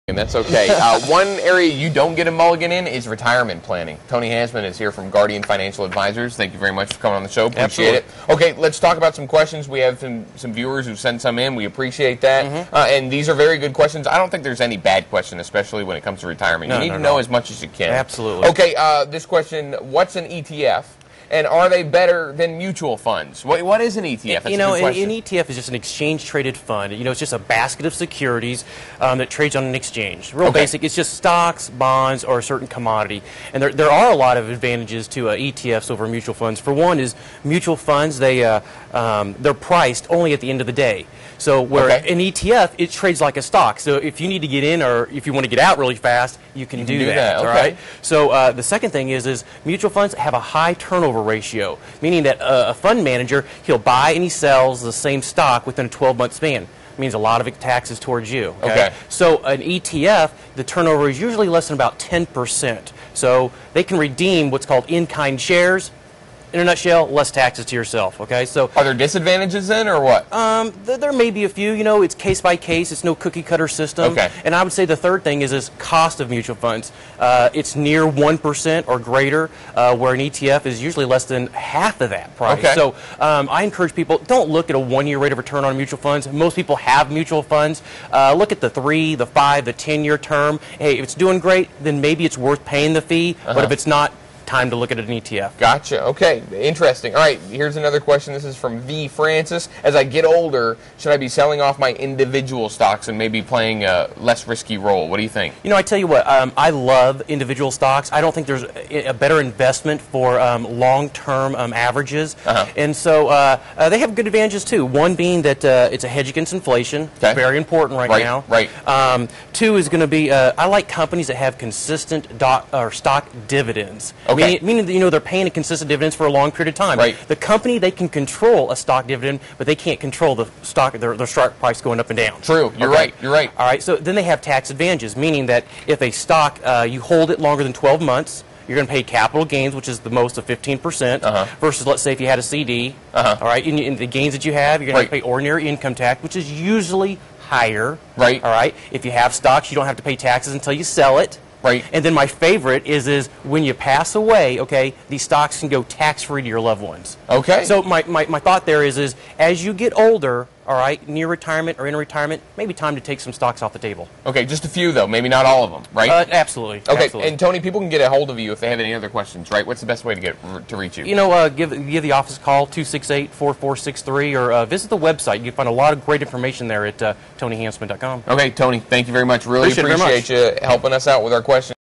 Guardian Financial - Tony Hansmann - Mutual Funds vs ETF's
Tony Hansmann on NBC (KNWA) discussing the benefits of ETF's over Mutual Funds and when it's time to reduce your stock risk.